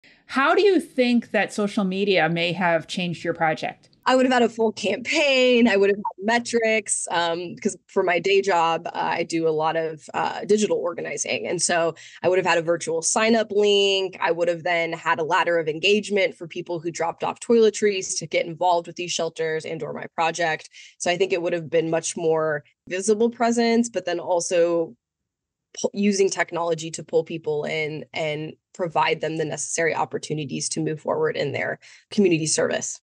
How to identify impact using data measurement for your Girl Scout Gold Award project
Afton, a former Girl Scout Gold Award recipient, shares her experience of completing her project in 2007, which involved collecting toiletries for domestic shelters in Knoxville, Tennessee. She reflects on the impact of her Girl Scout troop and her mother, who was the troop leader, in shaping her passion for community service and politics. Afton discusses how her Gold Award project influenced her career as a community organizer and state representative in Tennessee, where she advocates for voting rights and eliminating the grocery tax. She emphasizes the importance of pursuing projects that align with one's passion and encourages young women to make a difference in their communities.